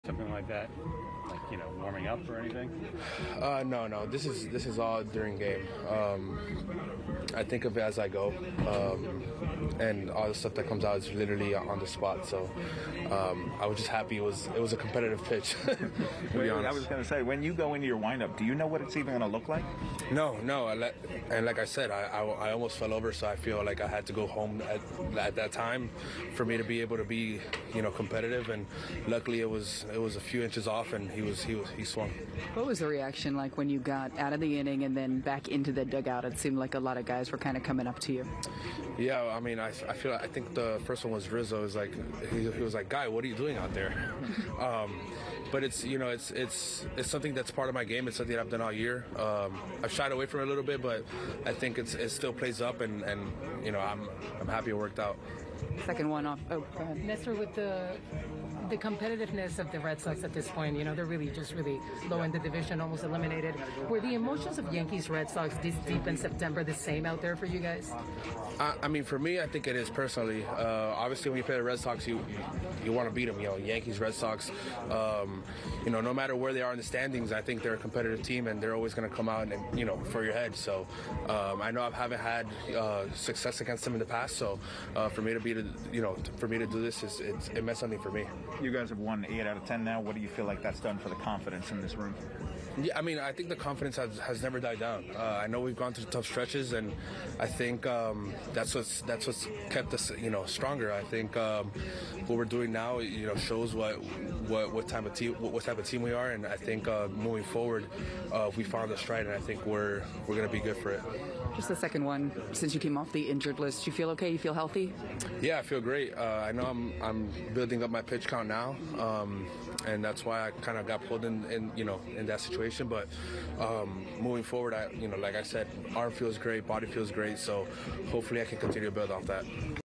Nestor Cortes earns win against Red Sox
Nestor Cortes picked up his 10th win of the season Wednesday night. The crafty southpaw pitched 5 innings of one-run baseball, including a unique delivery of a pitch to Rafael Devers.